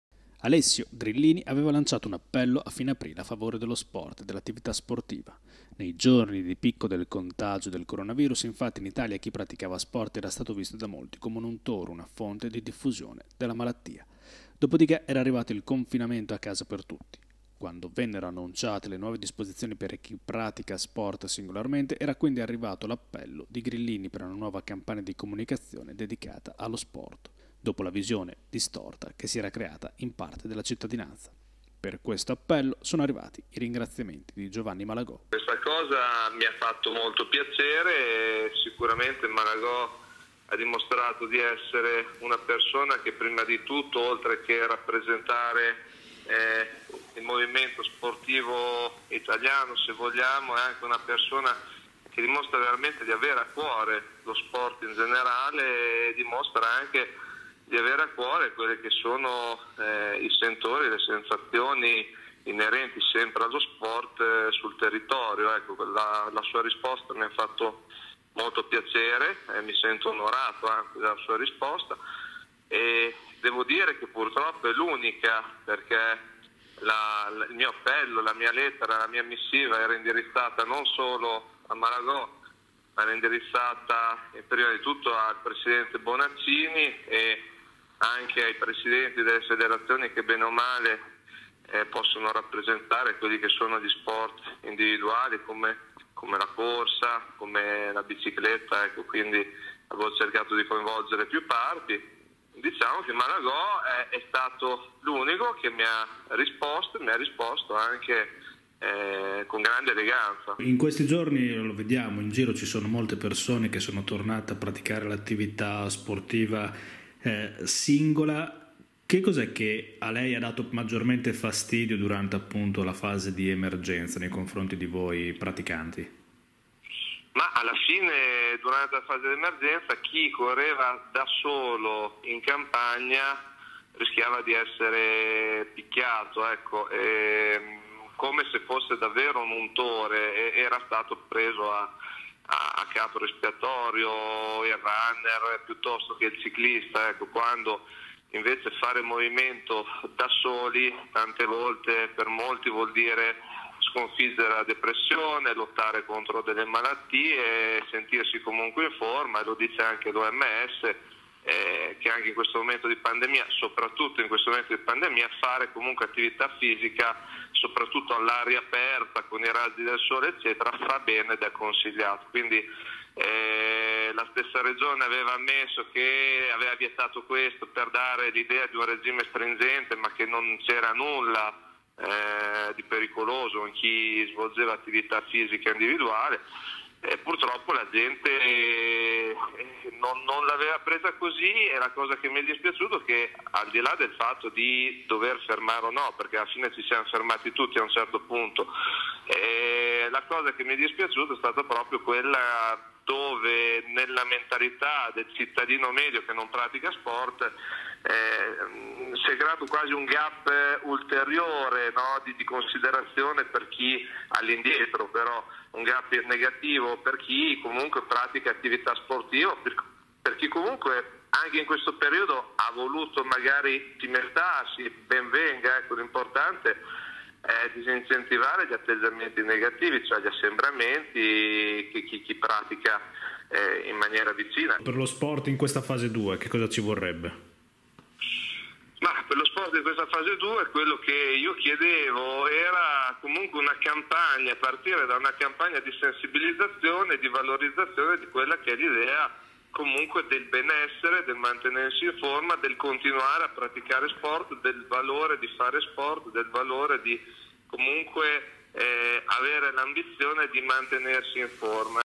Lo sport dopo il coronavirus: l'appello di Alessio Grillini e i ringraziamenti di Giovanni Malagò
Alessio Grillini aveva lanciato un appello a fine aprile a favore dello sport e dell’attività sportiva. Nei giorni di picco del contagio del coronavirus, infatti, in Italia chi praticava sport era stato visto da molti come un untore, una fonte di diffusione della malattia. Dopodiché era arrivato il confinamento a casa per tutti. Quando vennero annunciate le nuove disposizioni per chi pratica sport singolarmente era quindi arrivato l’appello di Grillini per una nuova campagna di comunicazione dedicata allo sport, dopo la visione distorta che si era creata in parte della cittadinanza. Per questo appello sono arrivati i ringraziamenti di Giovanni Malagò, presidente del Coni